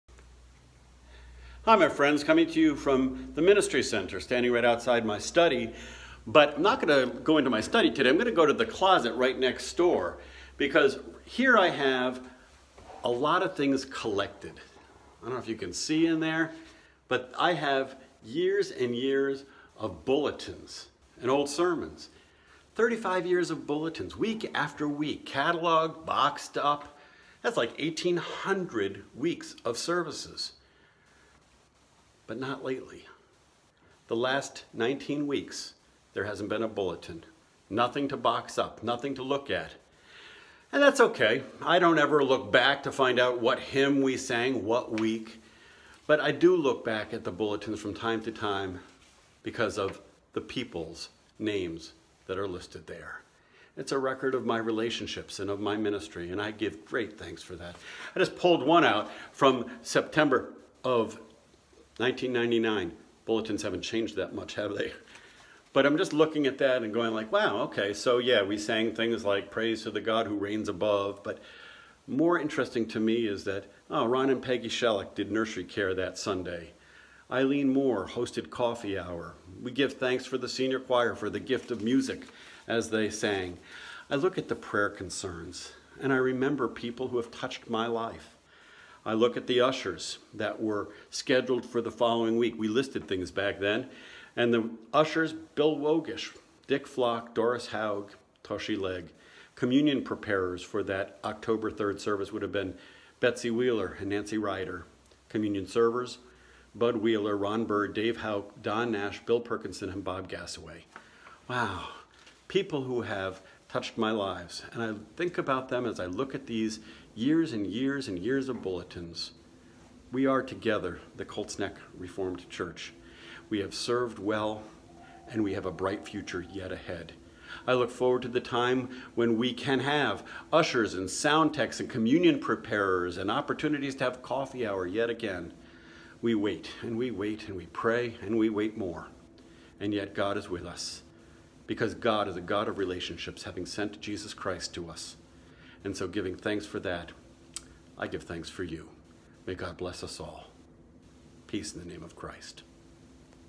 Colts Neck Reformed Church (CNRC) Moment with Scott 7/24/20
We are together, the Colts Neck Reformed Church. We have served well and we have a bright future yet ahead. I look forward to the time we can be together again. We wait, and we wait, and we pray - and we wait more. And yet, God is with us because God is a god of relationships having sent Jesus Christ to us.